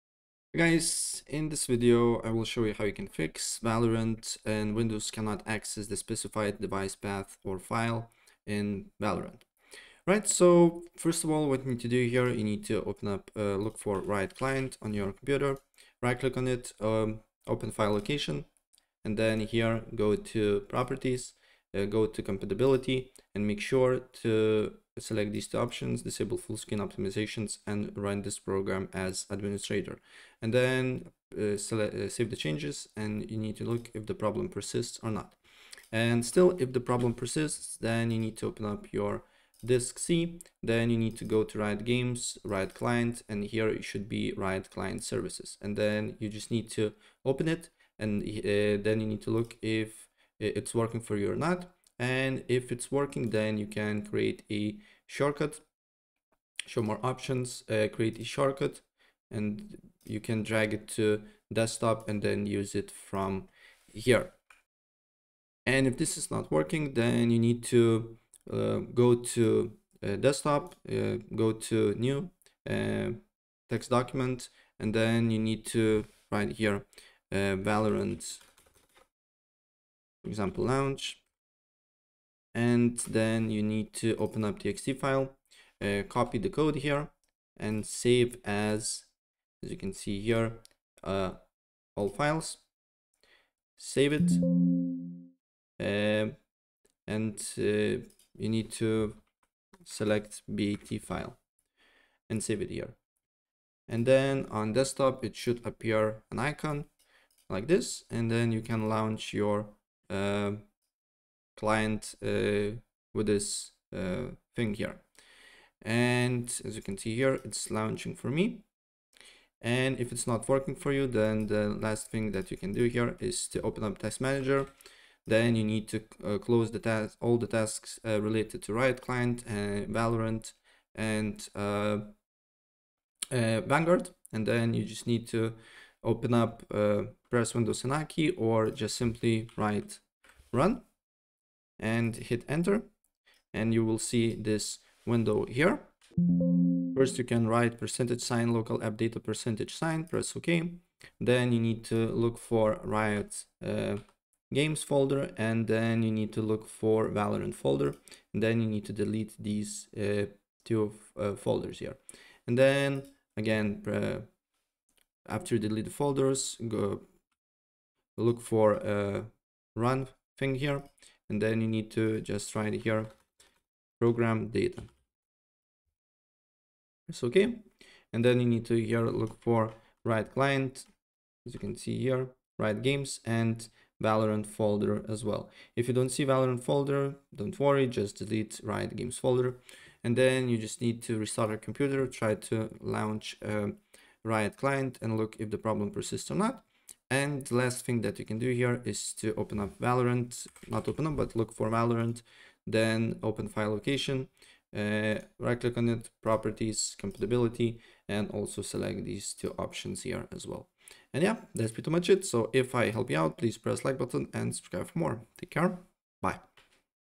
How To Fix Windows Cannot Access The Specified Device Path or File on Valorant 2024
In this video I will show you How To Fix Windows Cannot Access The Specified Device Path or File on Valorant 2024 It's really easy and it will take you less than a minute to do it!

Copy this command: "C:\Riot Games\Riot Client\RiotClientServices.exe" --launch-product=valorant --launch-patchline=live